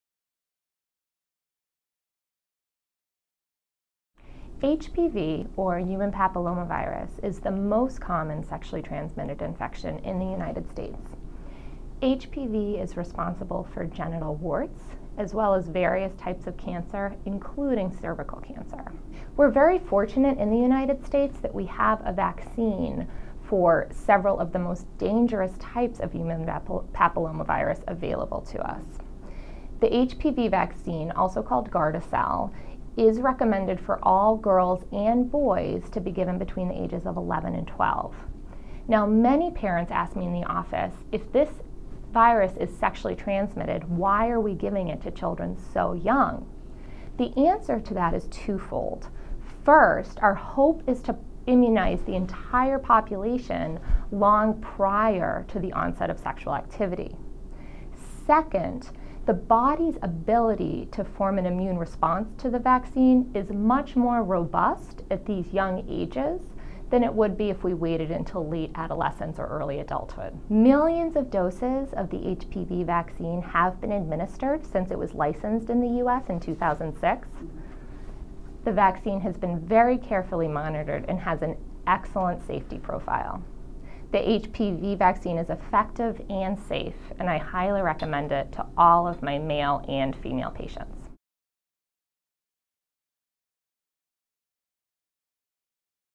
HPV Vaccine More Effective Prior to Sexual Activity
How much do you really know about the human papiloma virus (HPV) and its vaccine? HPV is the most commonly sexually transmitted disease in the United States. However, the HPV vaccination can reduce the health risks associated with the virus from occuring. Anne Powell, MD, pediatrician at UMass Memorial Children's Medical Center, speaks about the importance of boys and girls receiving the HPV vaccination between the ages of 11 and 12 receive this vaccination. Why so young? Watch the segment to learn the benefits of receiving the vaccine early.